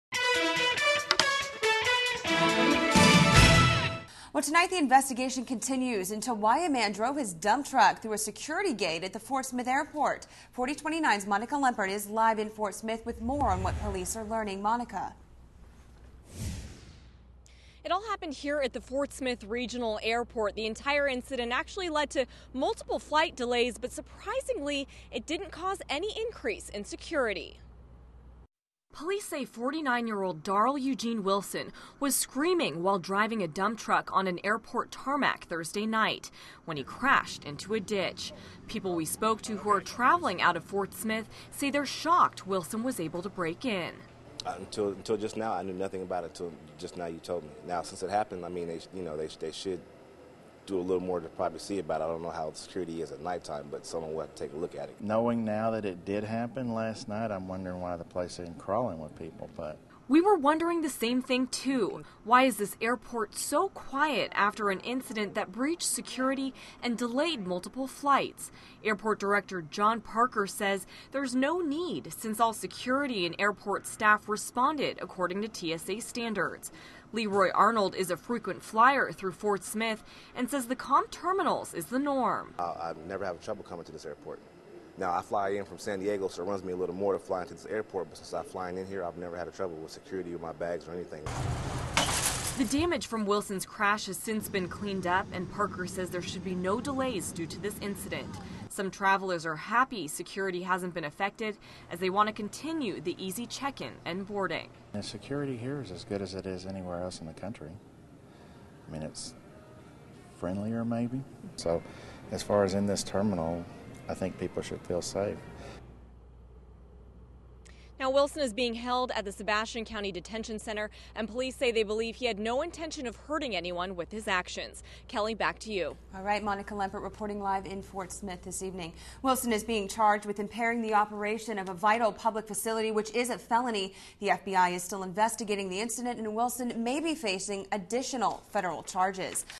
Fort Smith Airport Investigation Continues
The investigation continues into why a man drove his dump truck through a security gate at the Fort Smith Airport.